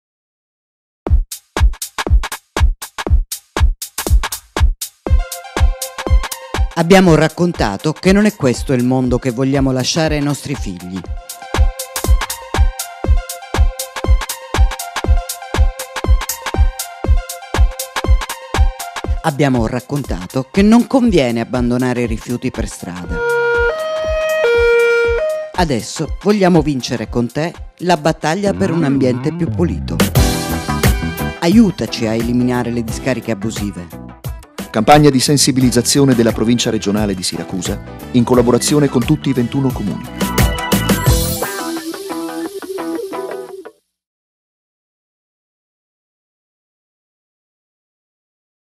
SPOT PROVINCIA TOLLERANZA ZERO 2012.mpg
Lo Spot della provincia Regionale di Siracusa.
Riguardante la campagnia Tolleranza Zero 2012